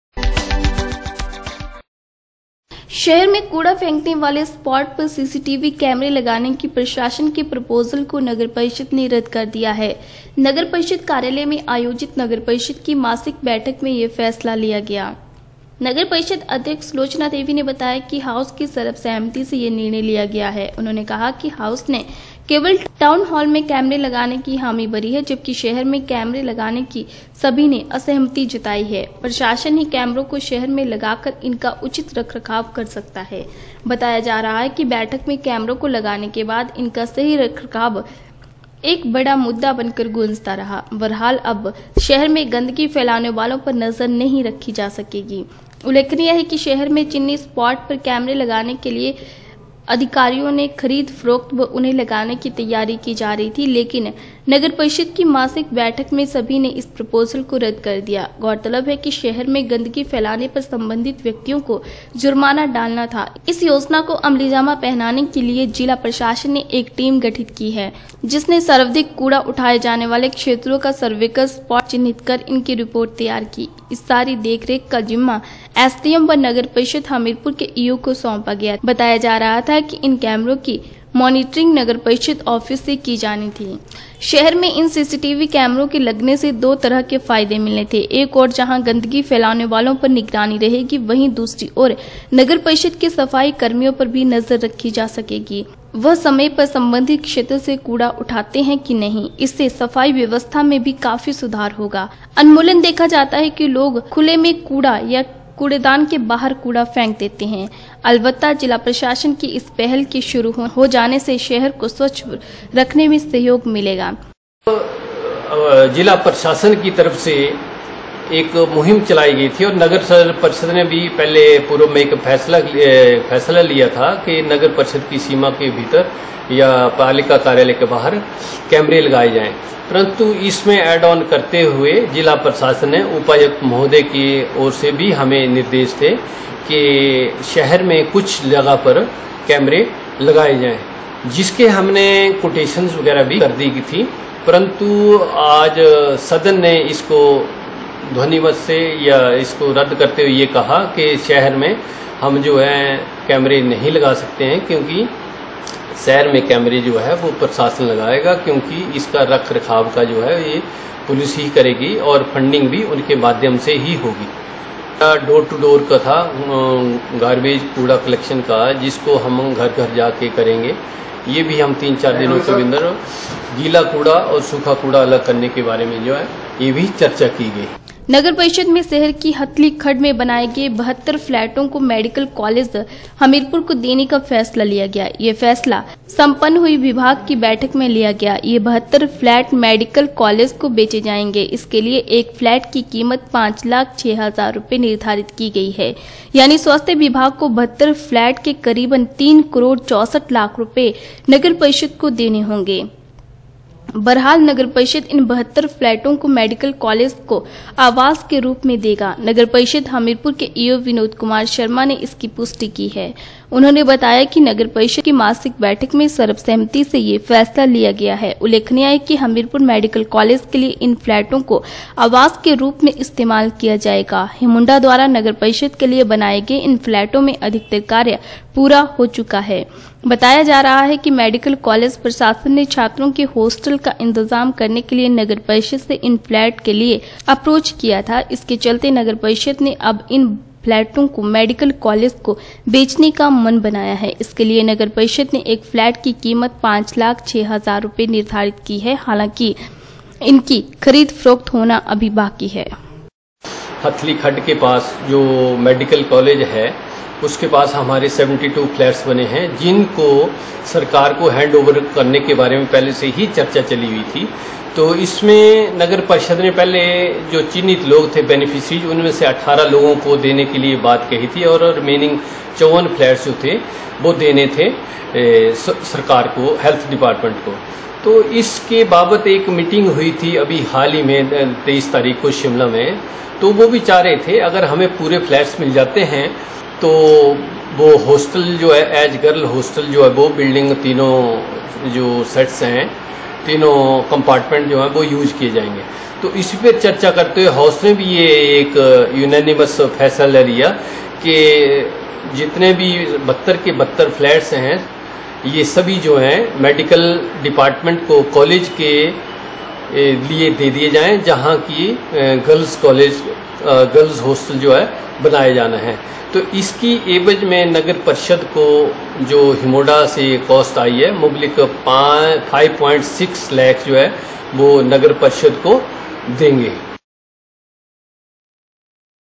कूड़ा फेंकने वाले स्पॉट्स पर सीसीटीवी कैमरे लगाने के प्रशासन के प्रोपोजल को नगर परिषद ने रद्द
शहर में कूड़ा फेंकने वाले स्पॉट्स पर सीसीटीवी कैमरे लगाने के प्रशासन के प्रोपोजल को नगर परिषद ने रद्द कर दिया है।  नगर परिषद कार्यालय में आयोजित नप की मासिक बैठक में यह फैसला लिया गया है। नगर परिषद अध्यक्ष सुलोचना देवी ने बताया कि हाउस की सर्वसहमति यह निर्णय लिया गया है। उन्होंने बताया कि हाउस ने केवल टाउन हाल में कैमरे लगाने को हामी भरीए जबकि शहर में कैमरे लगाने की सभी ने असहमति जताई है। प्रशासन ही कैमरों को शहर में लगाकर इनका उचित रखरखाव कर सकता है।